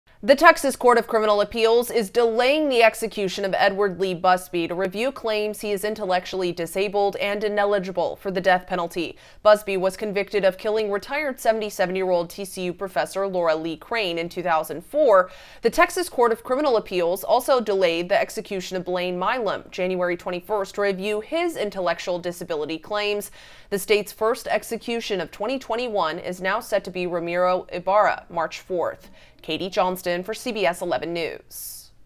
Texas Execution Of Edward Lee Busby Halted Over Disability Claims
A Texas appeals court has delayed a second execution this year to review claims that an inmate is intellectually disabled and thus ineligible for the death penalty. Katie Johnston reports.